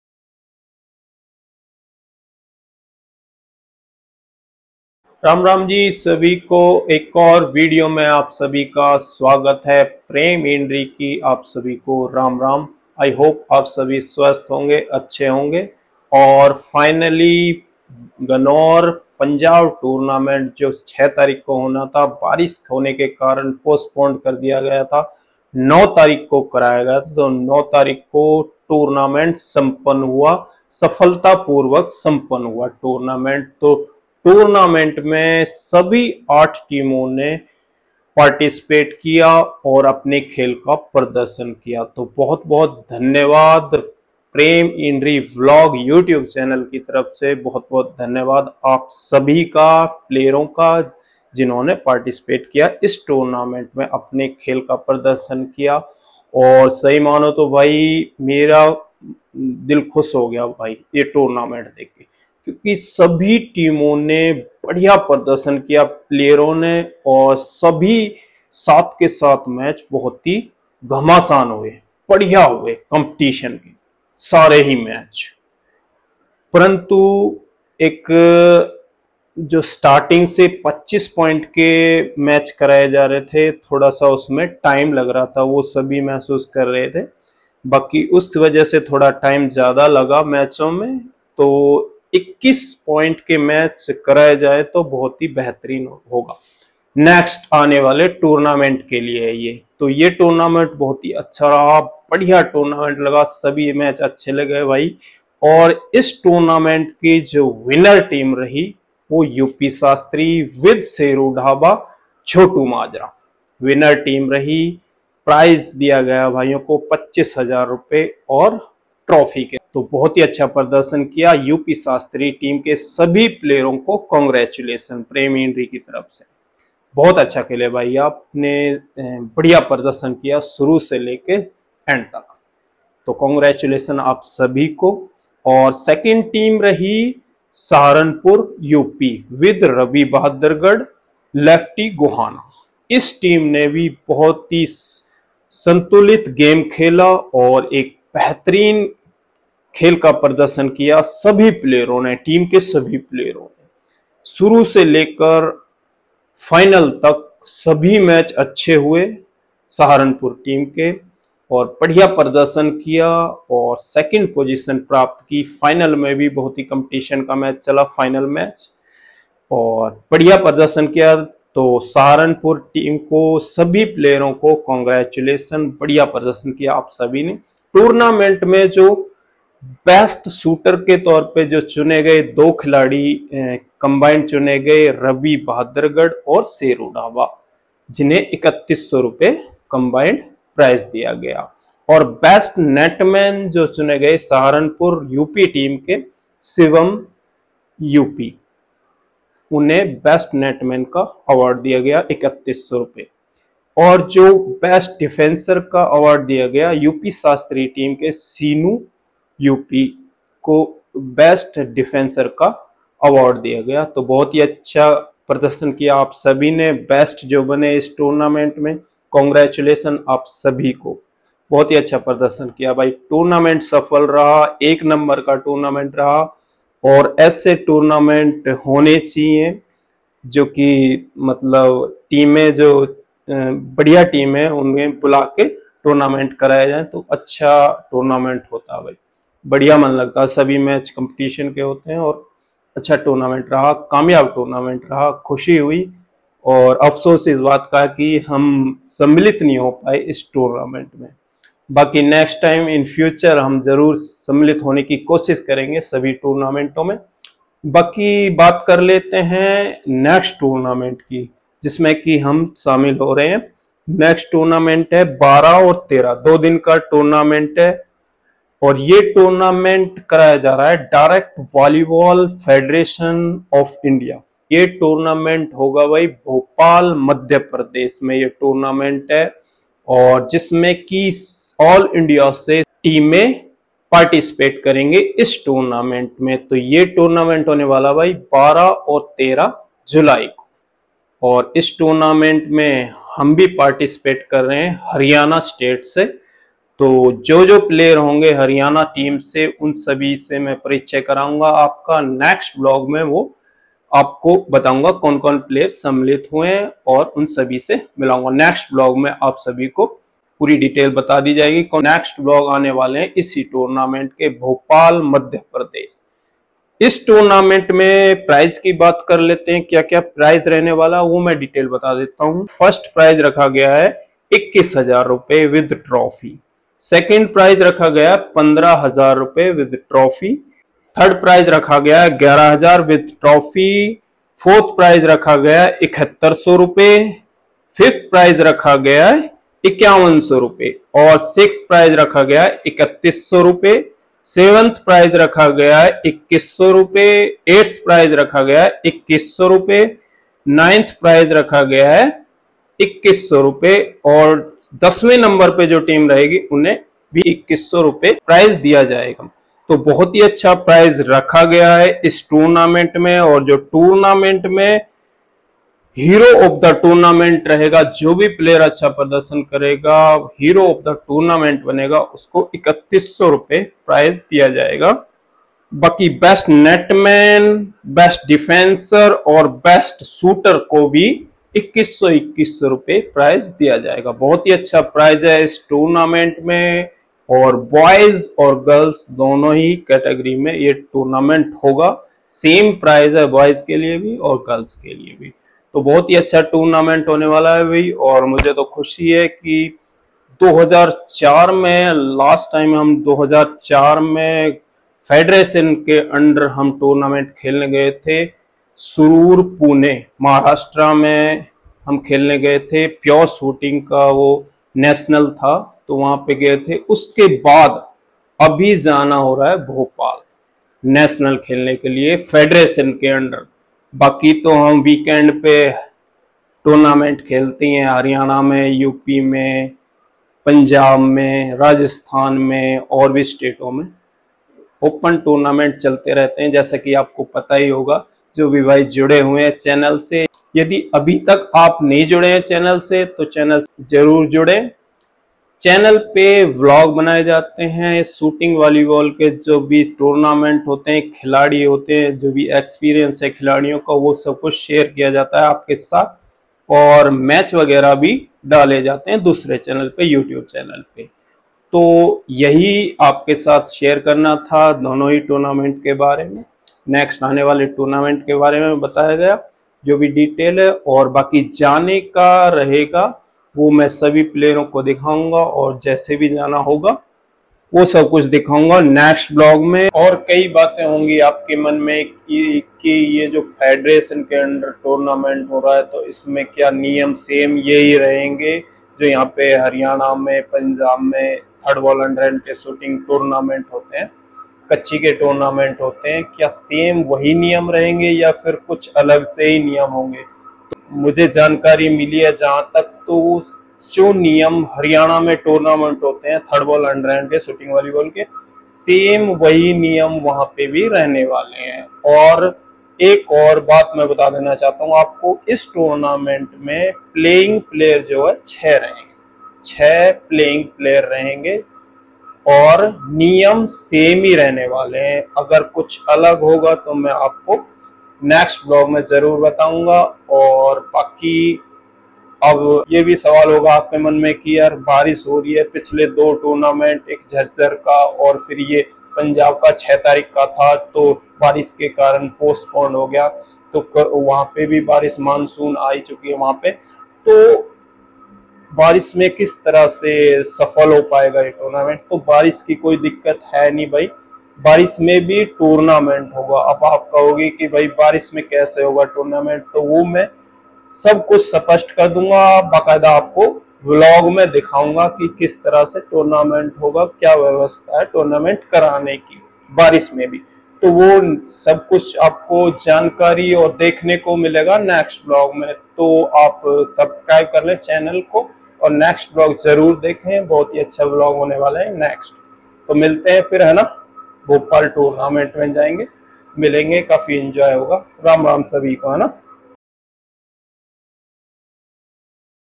National Championship at Bhopal MP, नियम Same ही रहेंगे 🏐Finally Tour. हो गया ओर कामयाब रहा👌🏐🙏
Telegram ID
All India Underhand Volleyball